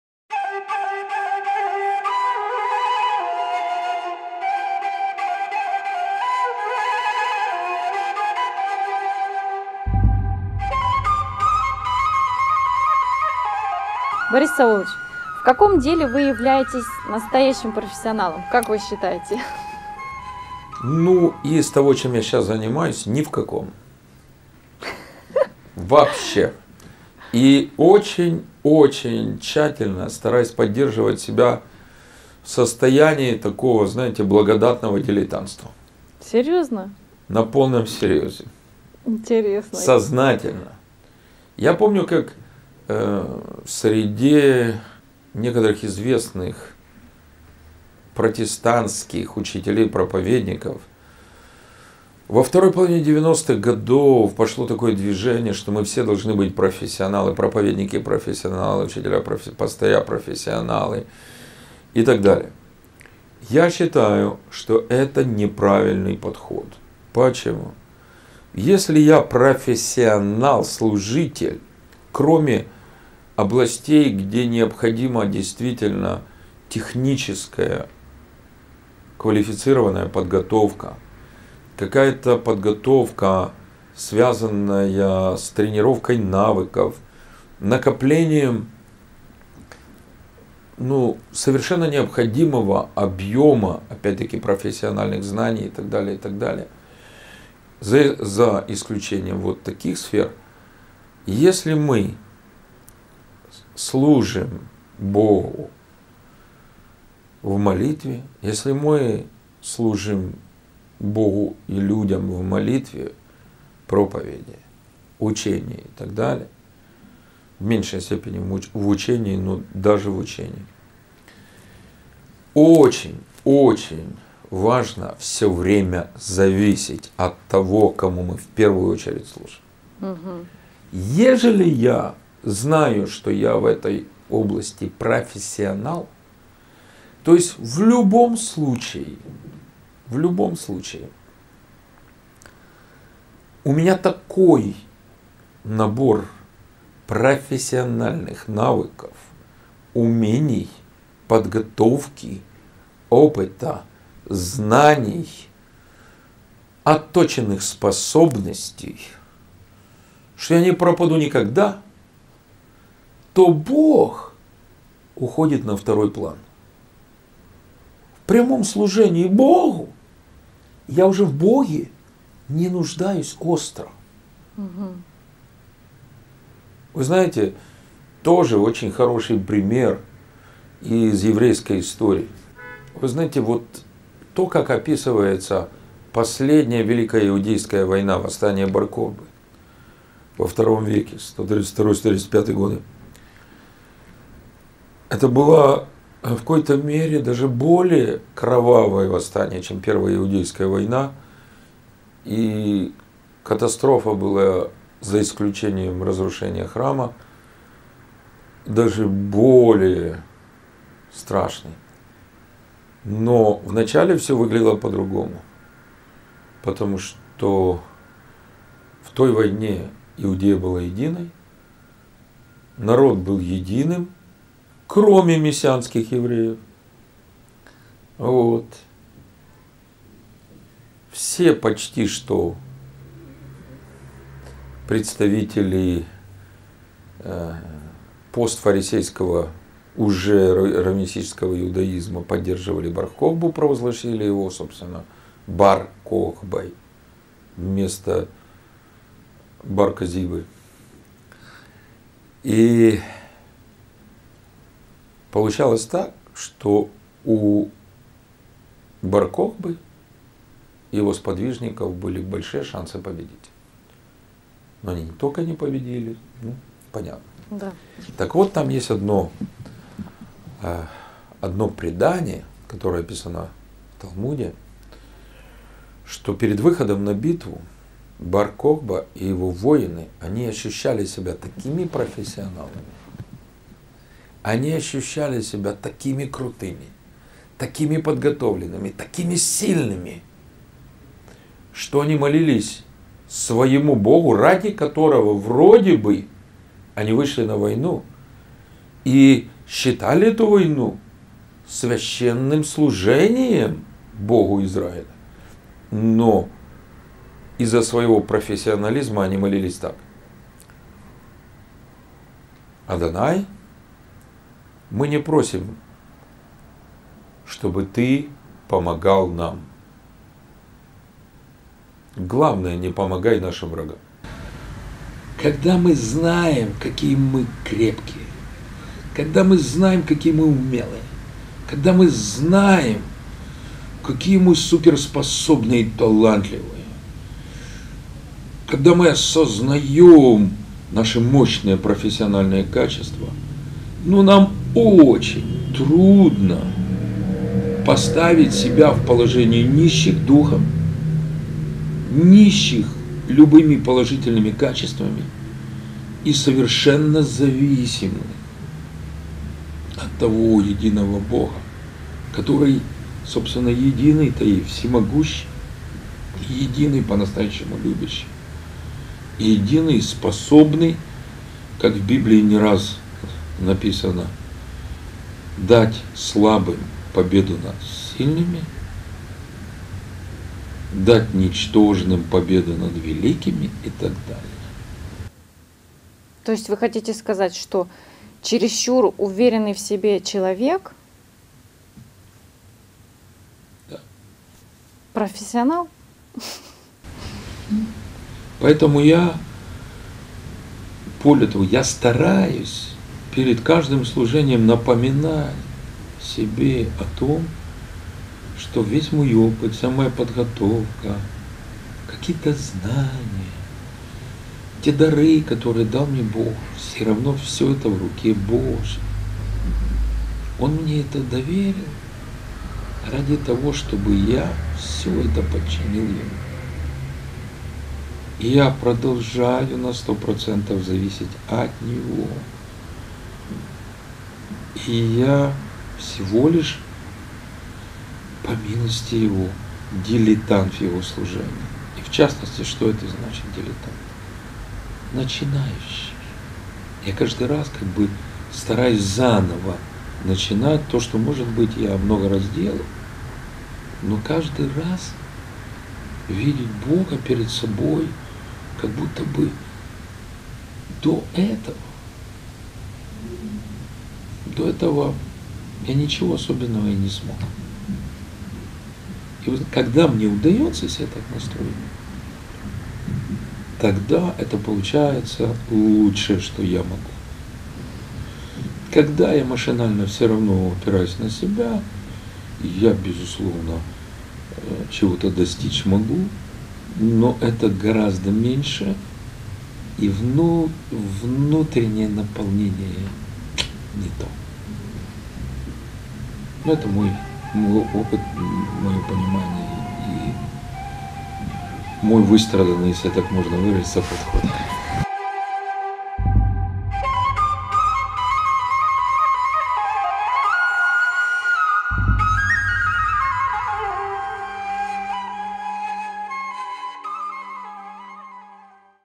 Почему опасно быть профессионалом в служении?
"Борис Саулович, в каком деле Вы являетесь настоящим профессионалом? Как Вы считаете?"
- Я помню, как в среде некоторых известных протестантских учителей и проповедников, во второй половине девяностых годов пошло такое движение, что "мы все должны быть профессионалами"...
Ответ старшего раввина КЕМО, Бориса Сауловича Грисенко.